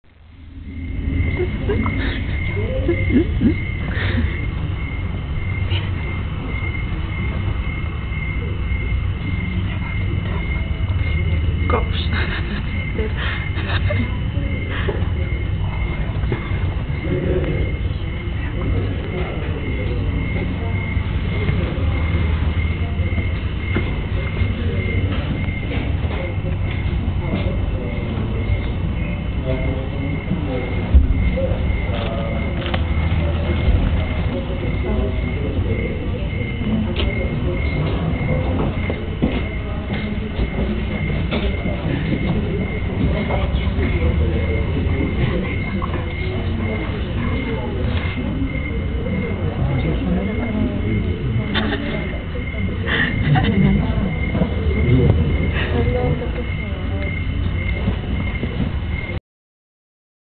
Tatu and Moja
Baby Gorillas at the Prague Zoo, Feb 09